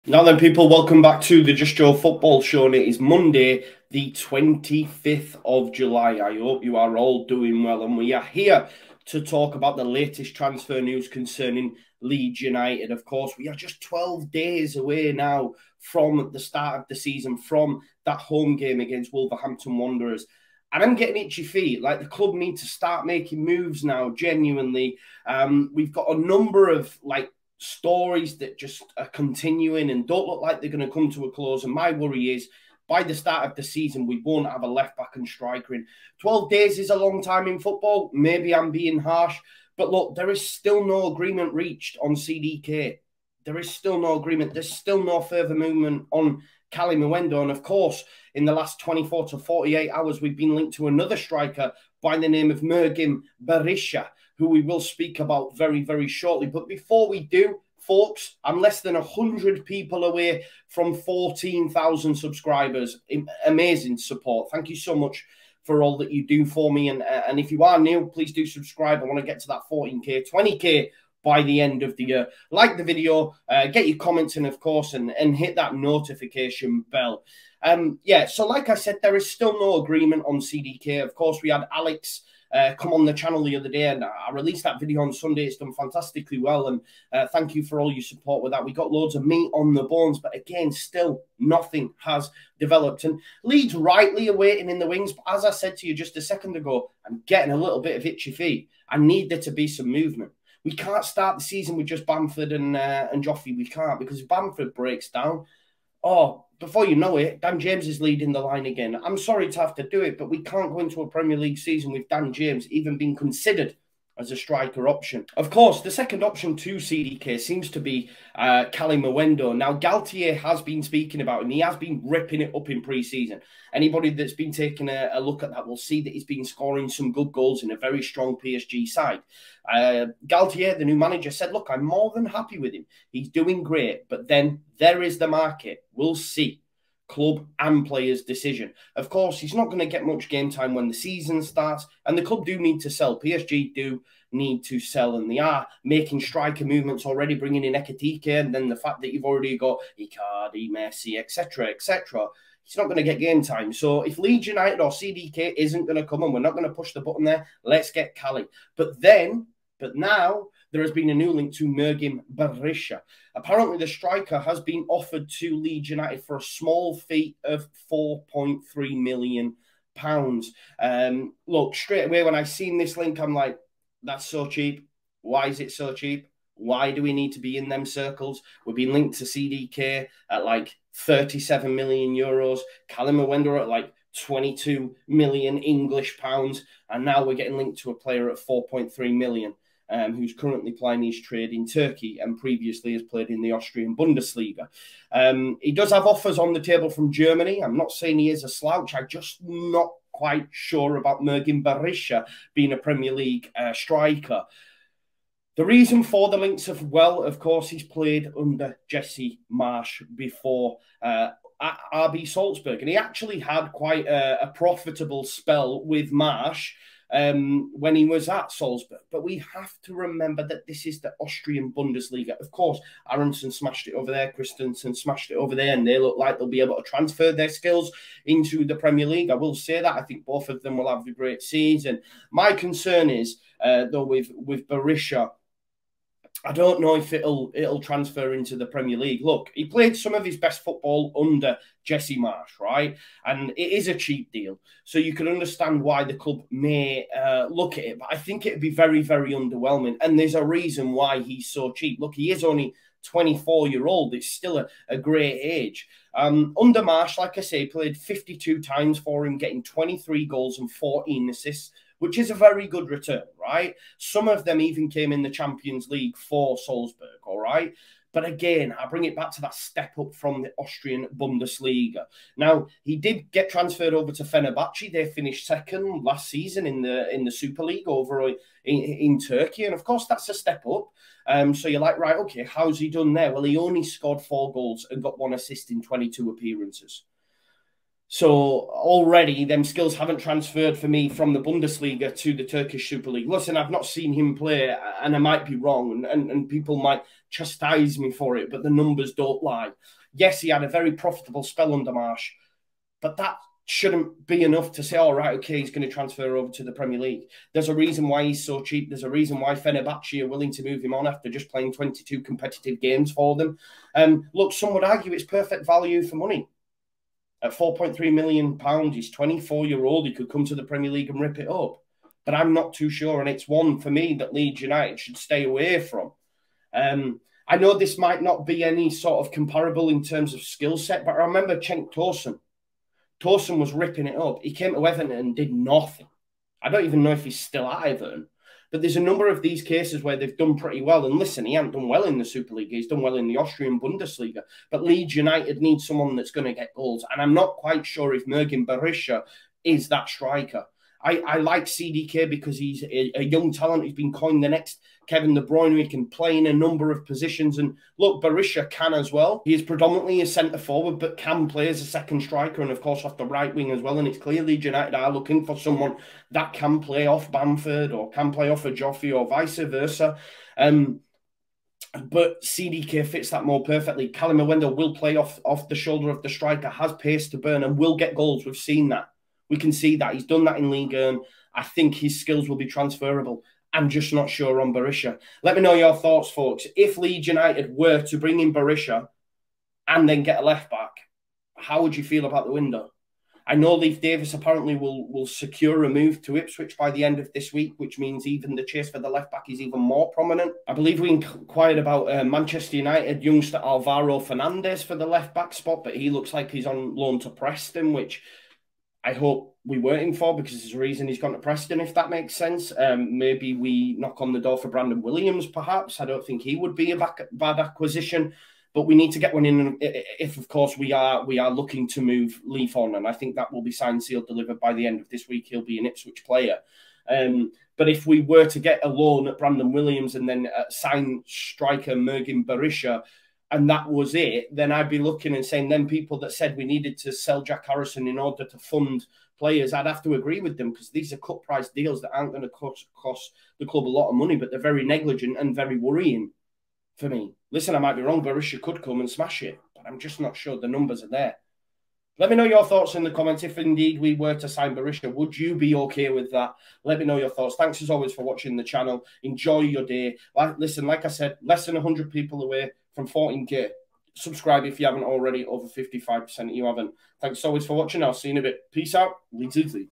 Could Leeds REUNITE Berisha with Marsch⁉️ Would it be a GOOD MOVE⁉️
Leeds United ready four-year contract for Mergim Berisha, £4.3m needed to seal transfer